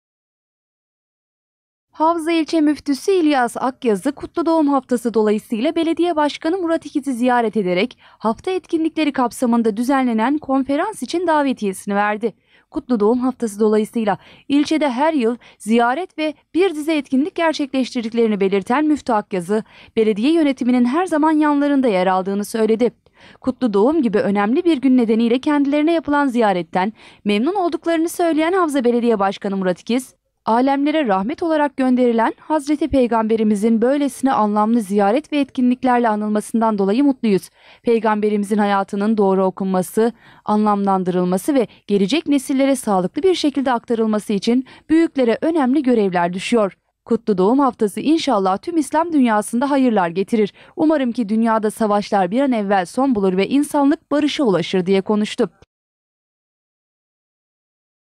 Büyüklere önemli görevler düşüyor
Havza İlçe Müftüsü İlyas Akyazı'yı ağırlayan Belediye Başkanı Murat İkiz, "Peygamberimizin hayatının doğru okunması ve gelecek nesillere sağlıklı bir şekilde aktarılması için büyüklere önemli görevler düşüyor" dedi.